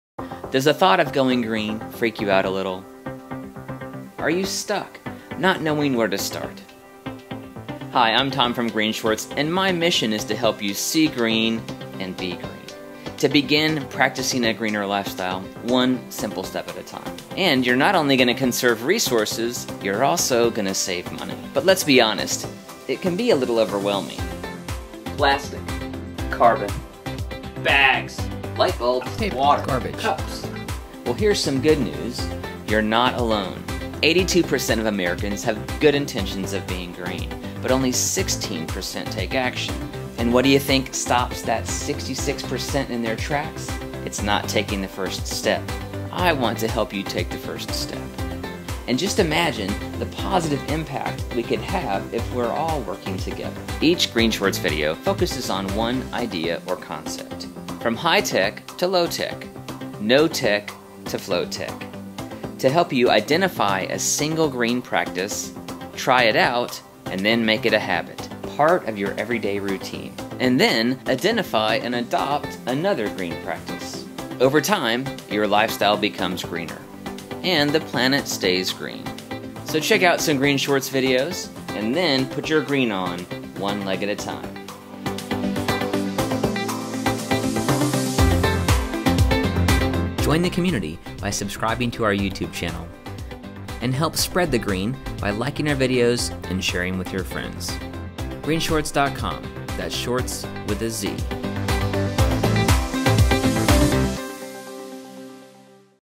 Going Green with GreenShortz
Green living can seem overwhelming if you look at it as a whole. The mission of GreenShortz is you help you see green in small, action-sized pieces. We create short videos, focused on one green idea or concept, with to stated goal of getting you started. Watch some videos and find one to start with. Put your green on, one leg at a time.